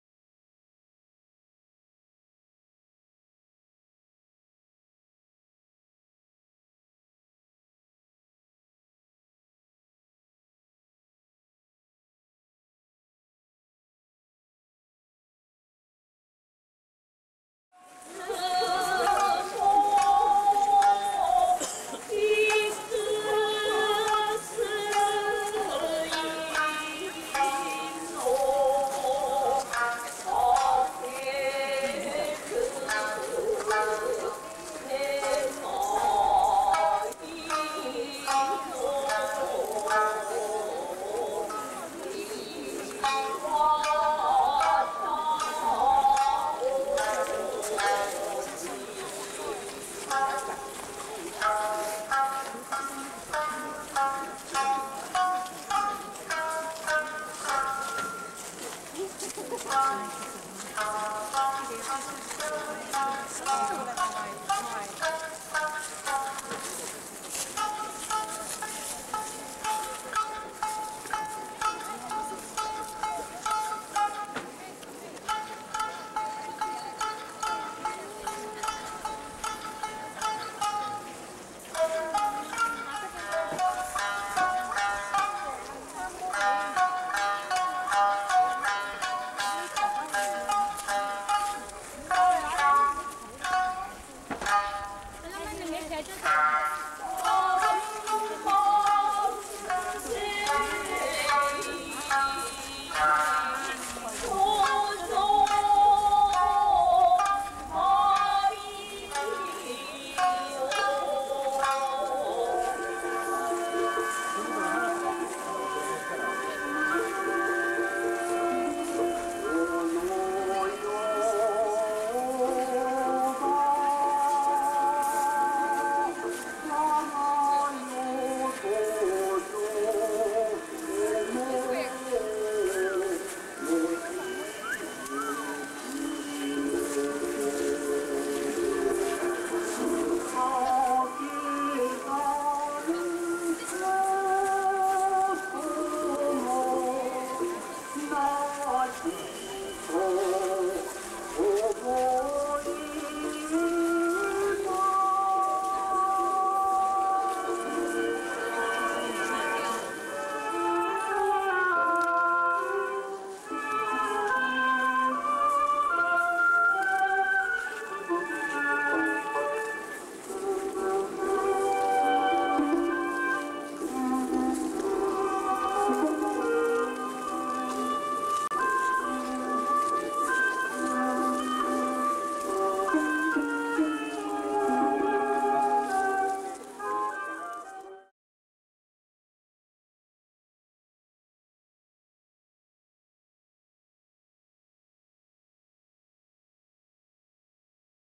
Geisha Dances at Heian Shrine
Heian Shrine, located in Japan's ancient capital of Kyoto, is the site for seasonal performances of one on this nation's most refined art forms, the elegant and serene geisha dances.